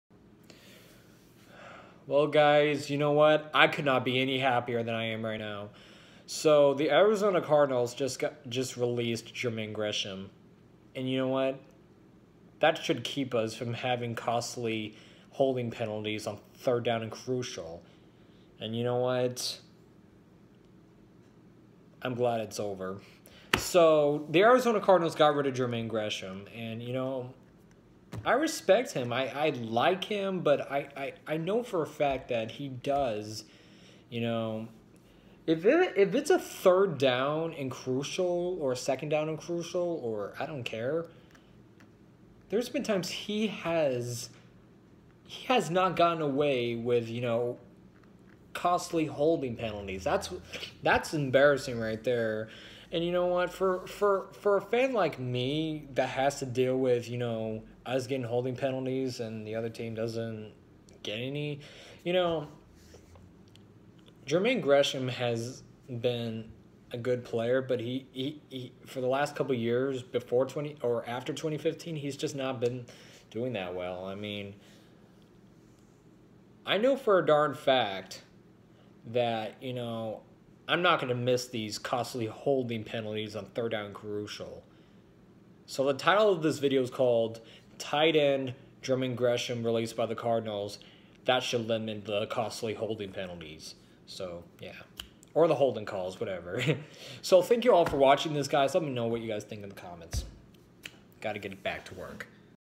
TE Jermaine Gresham released by the Cardinals... that should limit the costly holding penalties
Well we got rid of Jermaine Gresham. Let me know what you guys think in the comments. I know for a fact that he did have those costly holding penalties and I'm glad that's over.